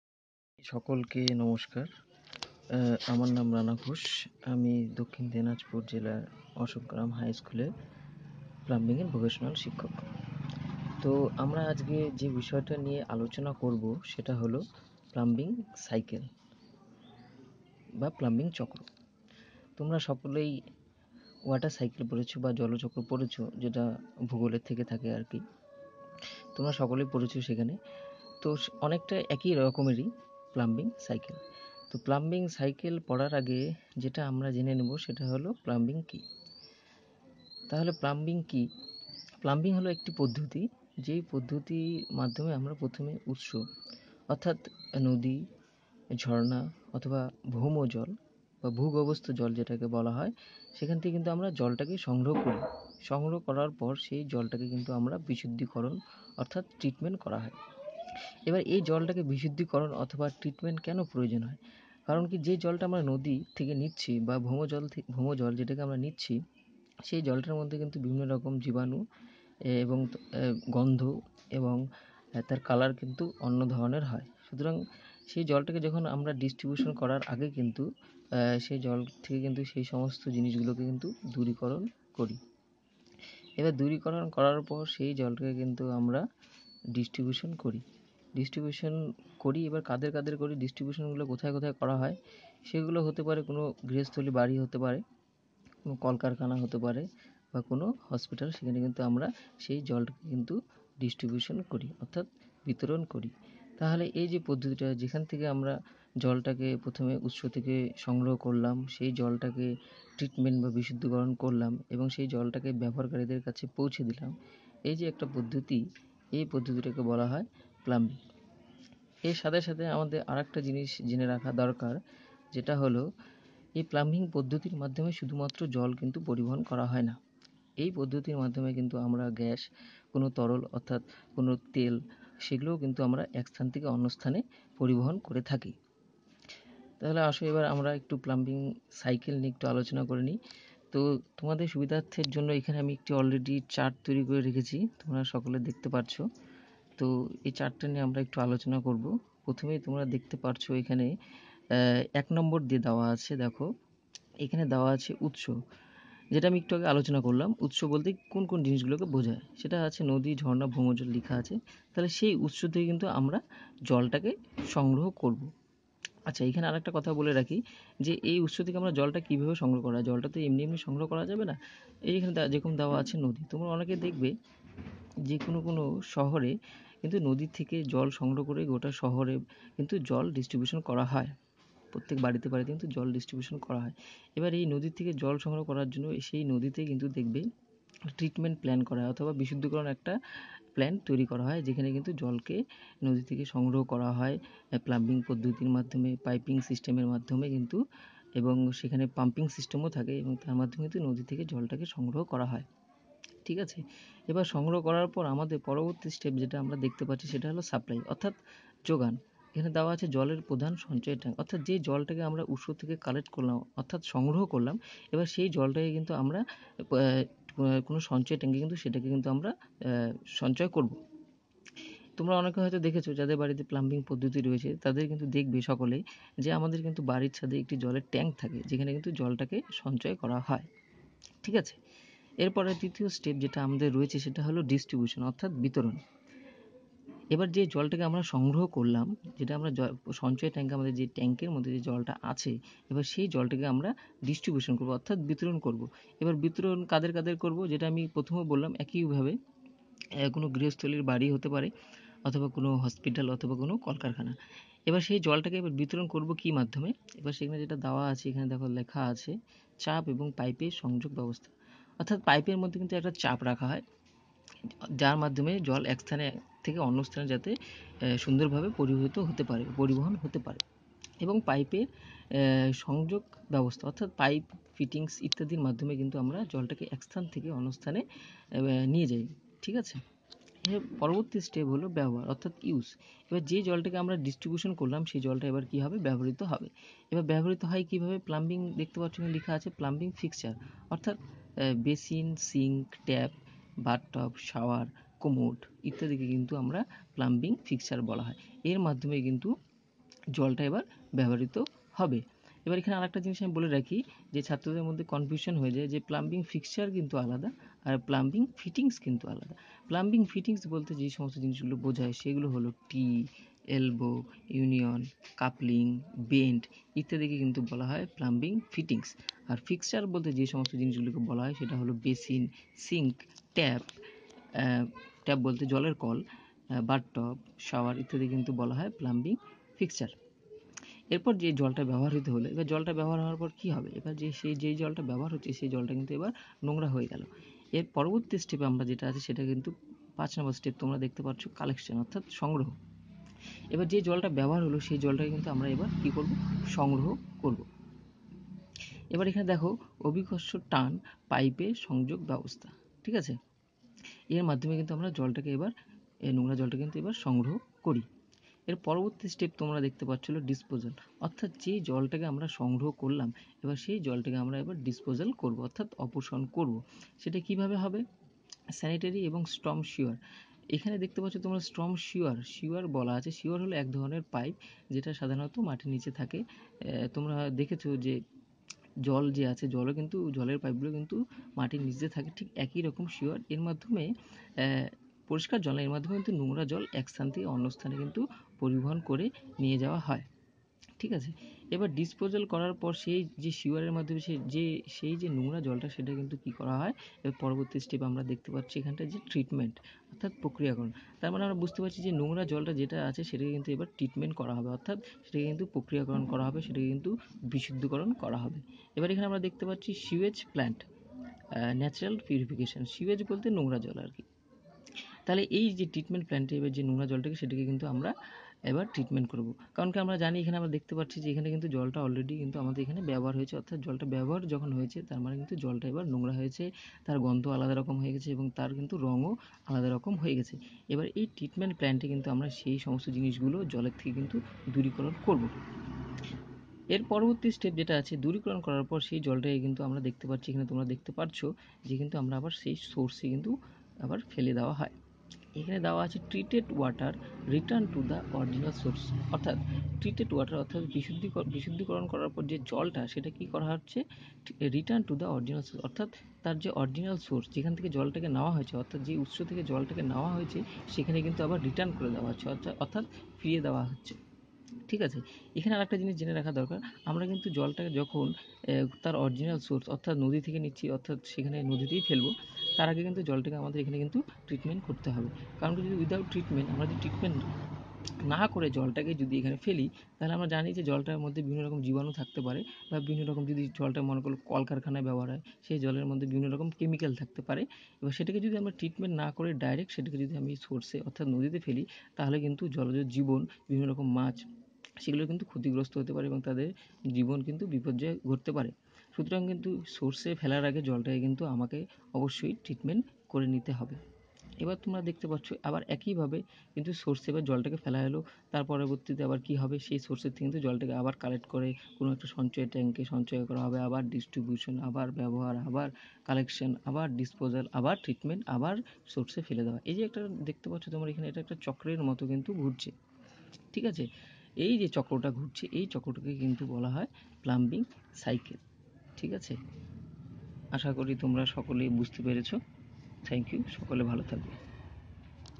Plumbing cycle by Rana Ghosh(Part-1)
In this video students will learn about Plumbing cycle.